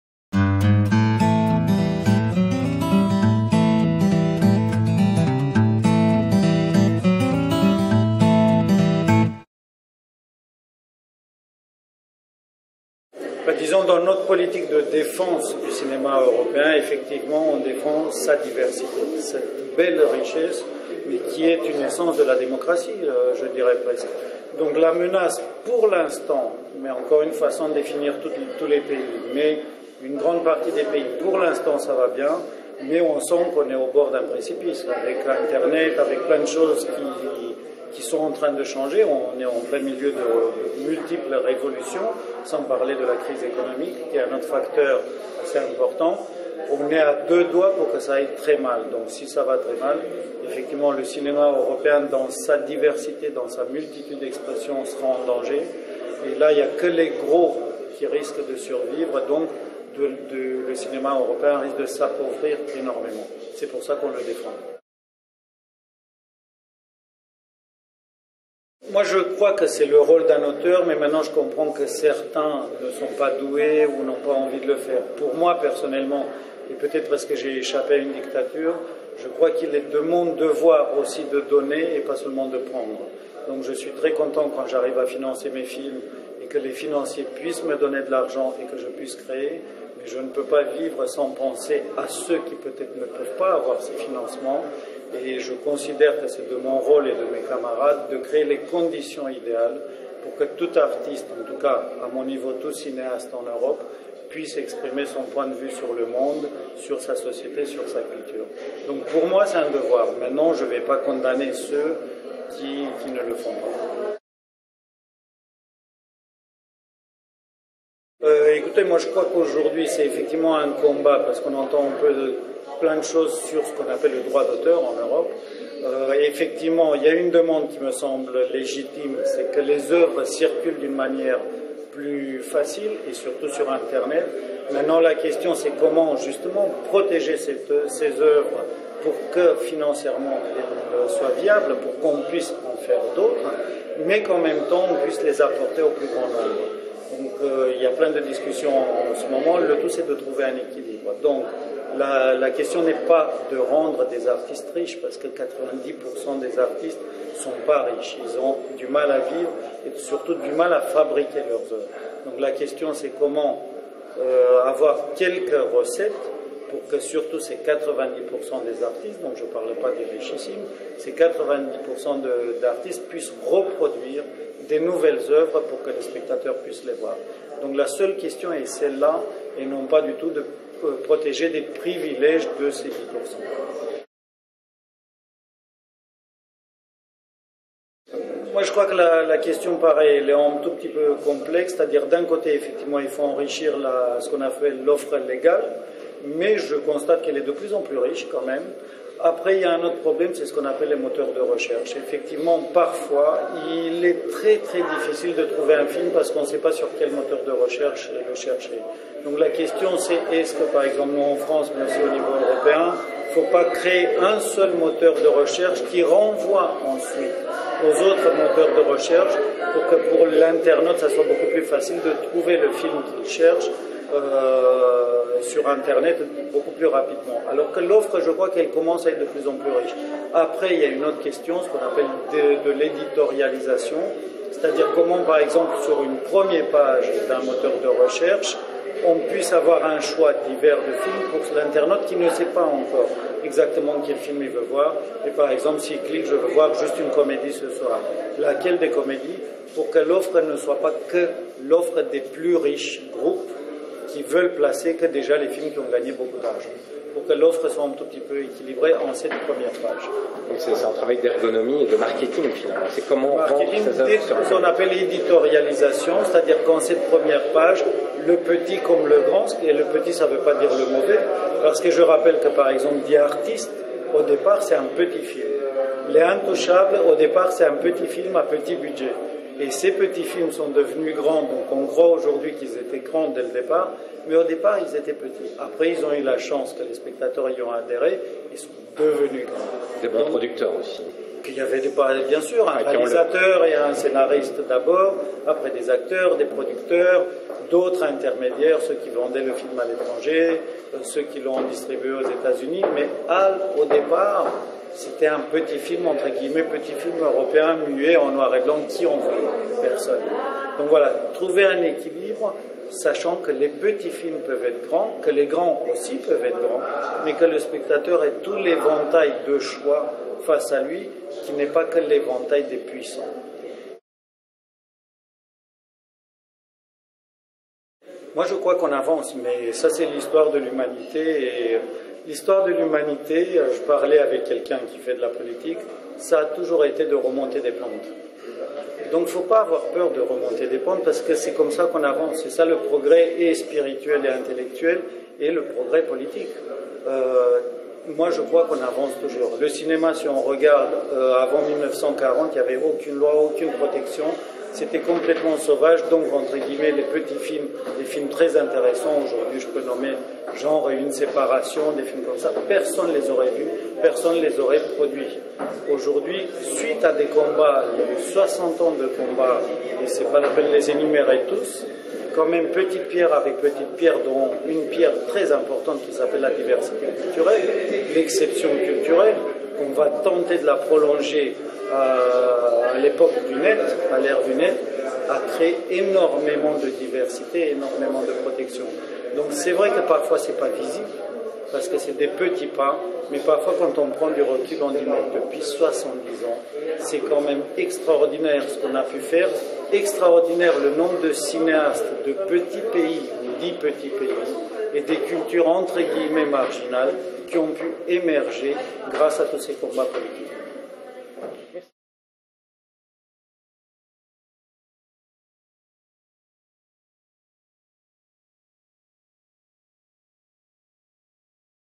Radu Mihaileanu - Director of The Concert - Les cinéastes invitent l'ami européen 2012
Cineuropa interviewed Romanian director Radu Mihaileanu (The concert, The Source) in Paris,where he took part in the 'Les cinéastes invitent l'ami européen' initiative 2012, organised by ARP and Cinéma des Cinéastes, in partnership with SAA and SACD.